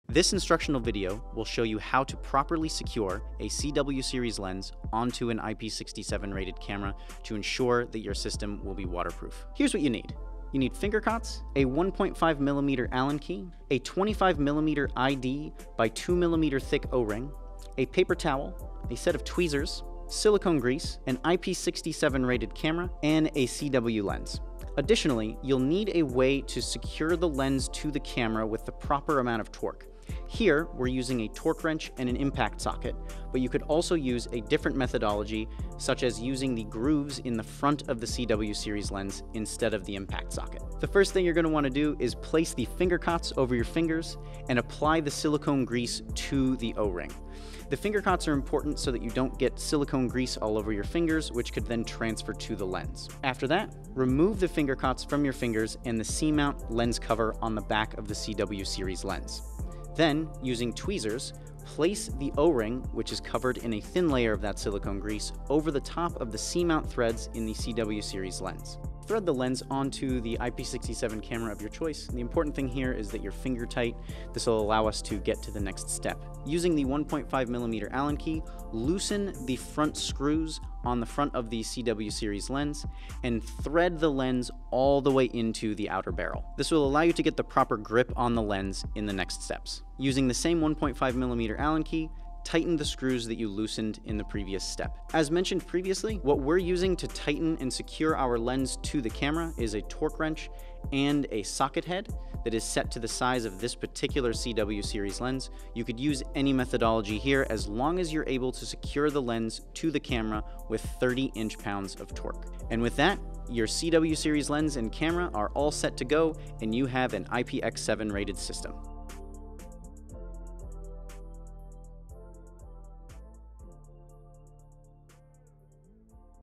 How to Mount the IPX7, IPX9K Cw Series Lens to a Waterproof Camera
Connecting the waterproof (IPX7 and IPX9K) Cw Series Lens to an ingress protected camera is easy. This video will walk you through the process.